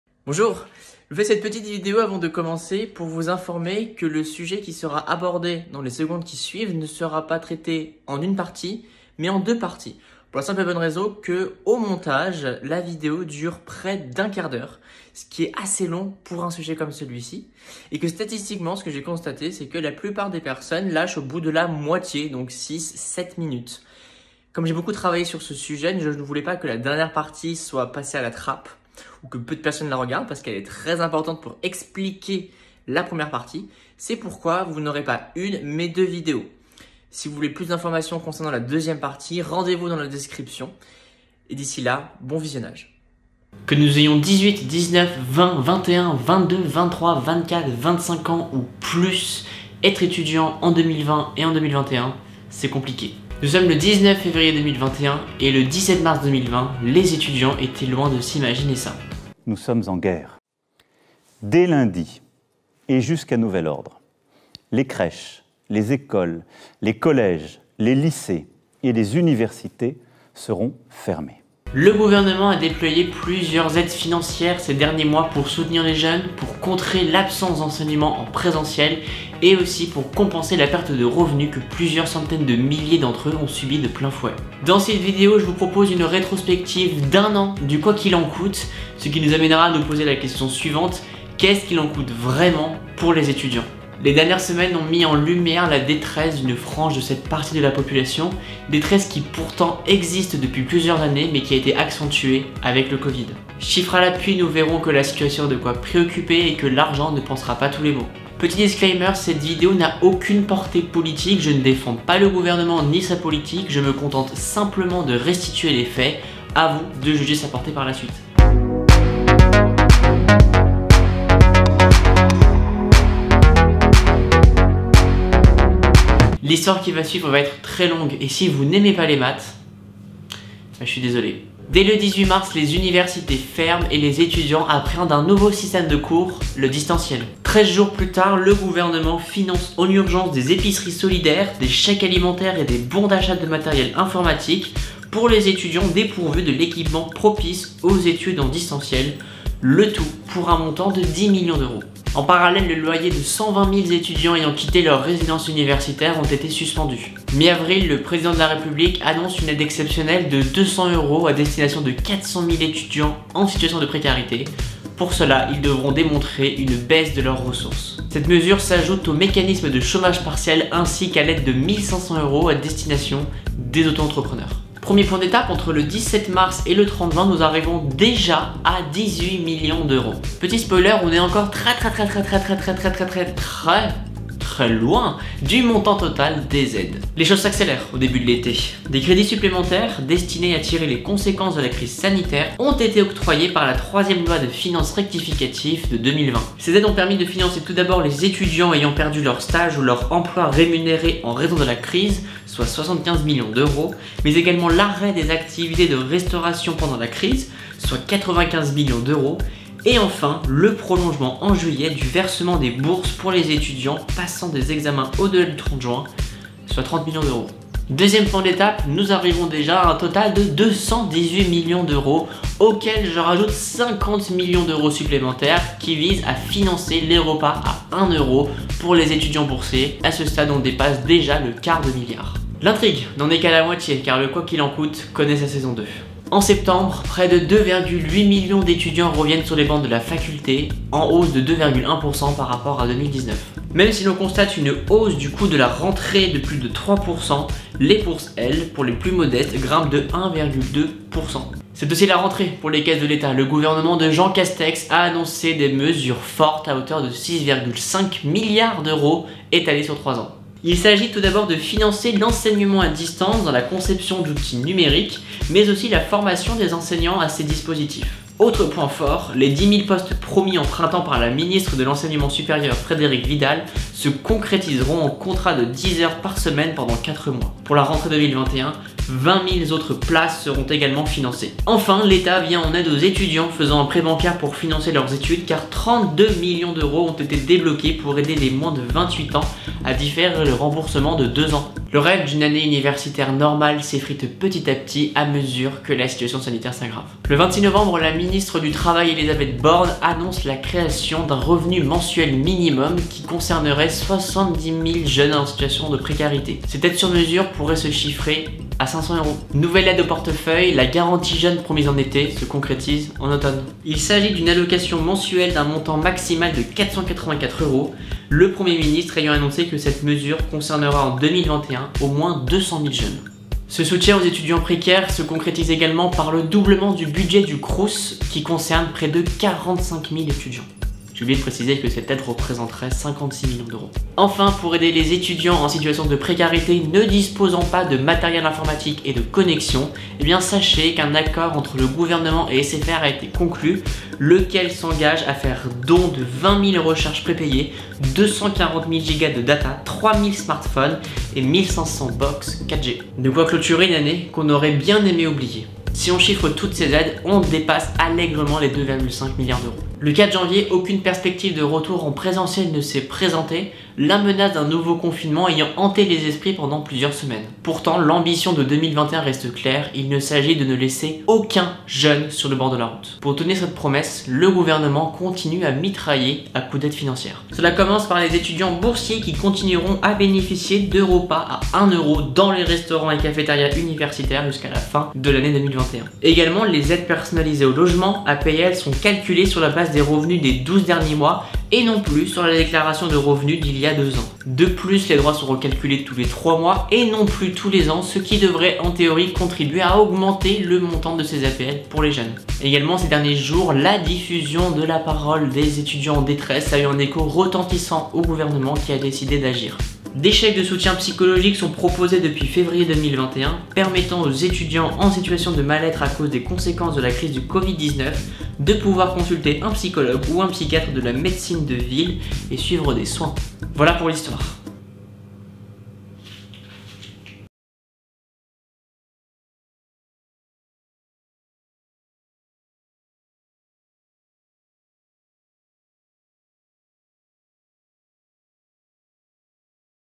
Jeunesse 2021 : le "quoi qu'il en coûte" étudiant — Décodeur #4 (partie 1/2)
😶 Avoir 18, 19, 20, 21, 22, 23, 24, 25 ans en 2021, ce n'est pas facile. Encore moins quand on est étudiant. Il y a presque un an, cette génération était loin de s'imaginer les répercussions de l'actuelle crise sanitaire sur leurs études. Pour remédier aux maux, le gouvernement a déployé un arsenal financier non négligeable tout au long de ces derniers mois. Le "quoi qu'il en coûte" peut-il vraiment sauver la jeunesse ? Éléments de réflexion dans cette série de vidéo.

➡️ La partie 2 : (BIENTÔT DISPONIBLE)

Présentation :
00:00 Information de dernière minute
00:43 Introduction
01:58 Jingle
02:06 La gestion de l'année universitaire 2020
03:52 Les appréhension de la rentrée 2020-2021
06:00 Les enjeux de 2021